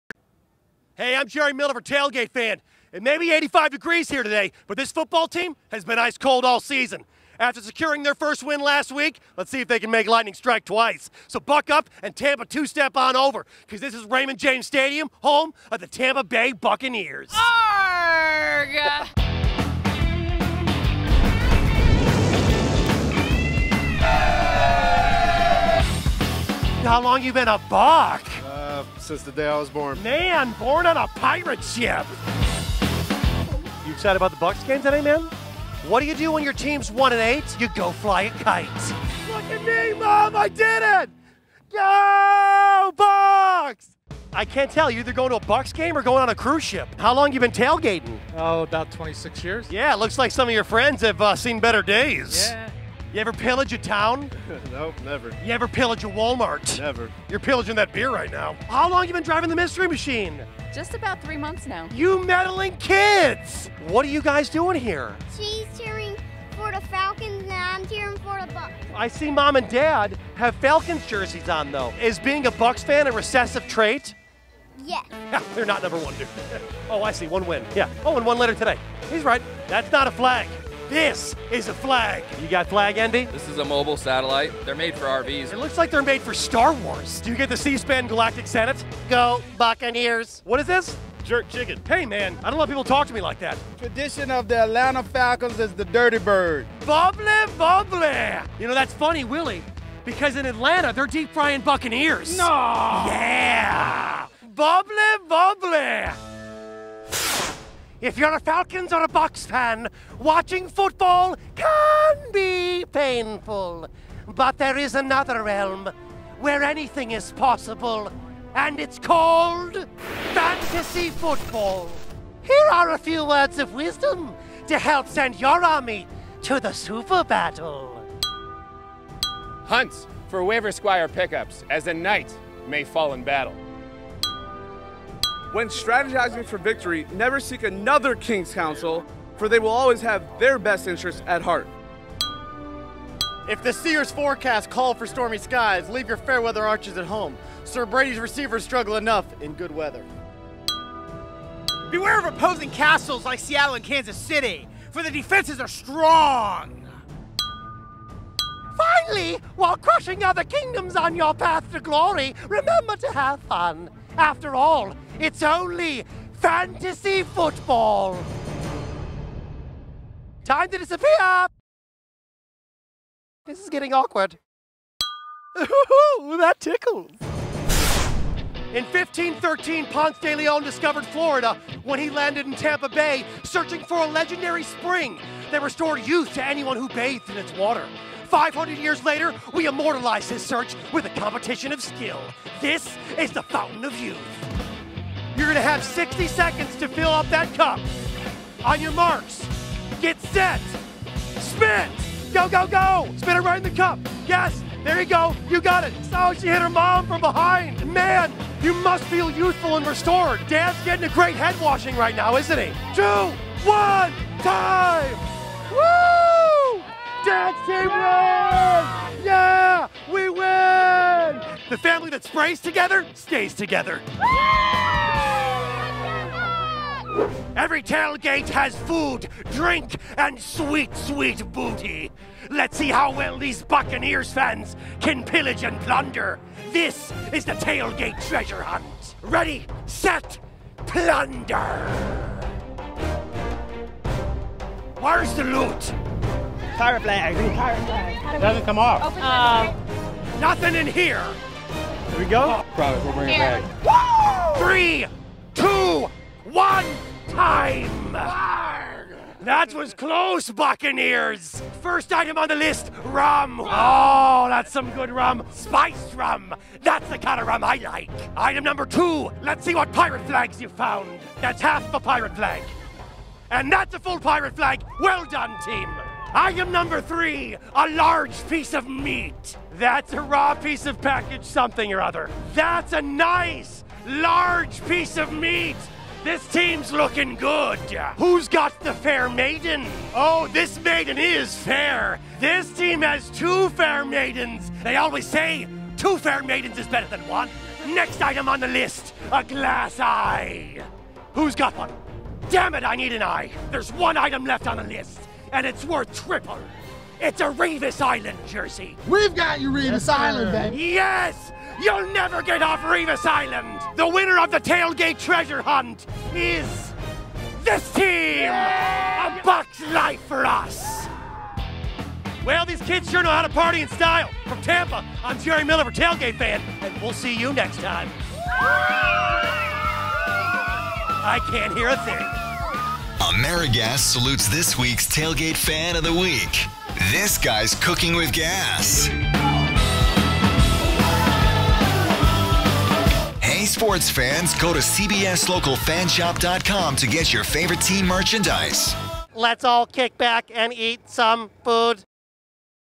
Tailgate Fan: Tampa Bay Buccaneers
Follow Jerry Miller as he challenges the Tampa faithful to a Tailgate Treasure Hunt and gives fantasy football advice to all you tailgate fans.

'Fan Sports Live' gives fans a behind-the-scenes look at sports through exclusive original programming, broadband in-studio shows and live telecasts!